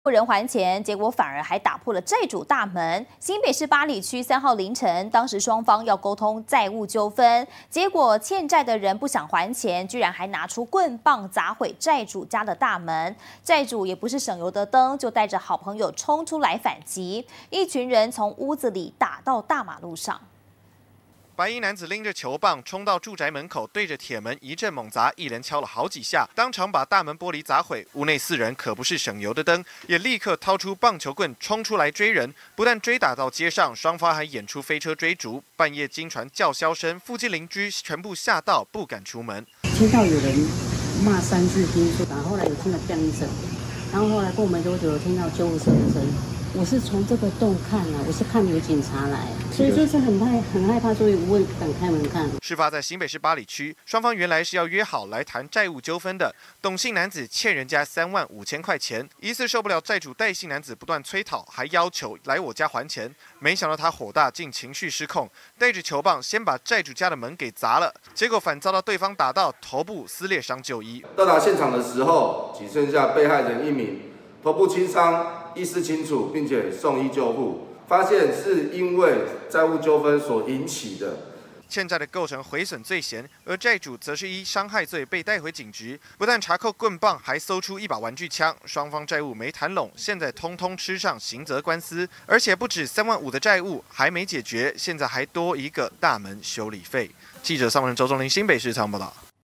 欠債男持球棒砸門債主撂3人反擊開戰
債務人還錢，反先打破債主大門。事情發生在新北市八里區，3號凌晨，當時雙方要溝通債務糾紛，但債務人不甘還錢，先拿棍棒砸毀債主大門，不過債主也不是省油的燈，帶著3名好友衝出來反擊，雙方一路從屋內，打到大馬路，嚇的鄰居都不敢出門。